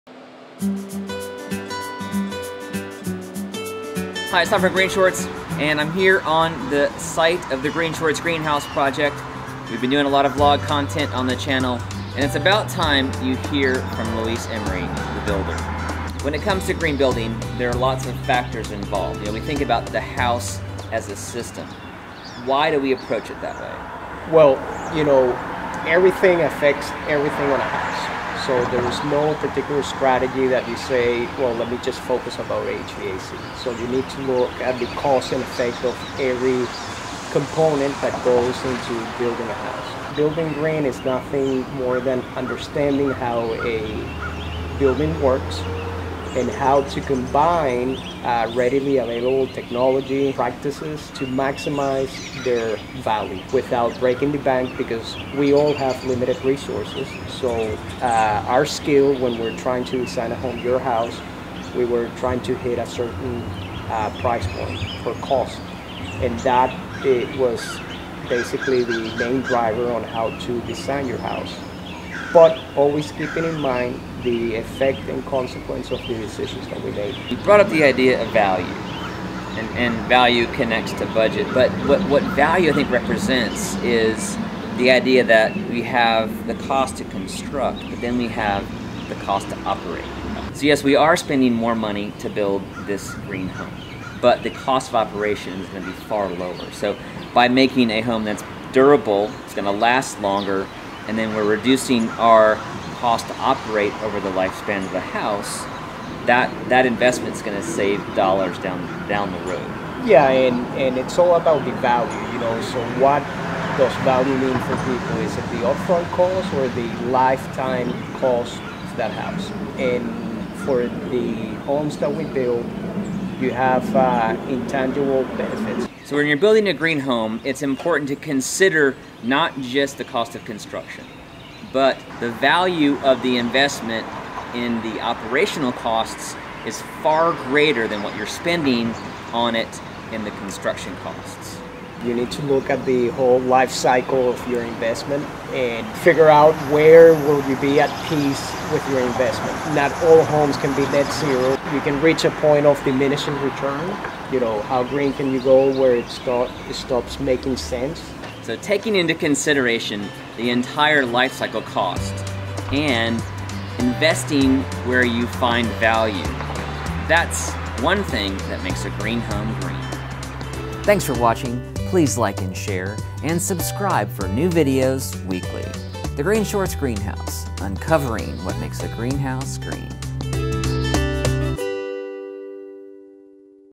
The "Value" Proposition | TECH 001
I discuss the importance of investing in green technology where "value" can be identified. When building a green home on a budget, you have to choose what "green" elements to spend money on, and what to not include.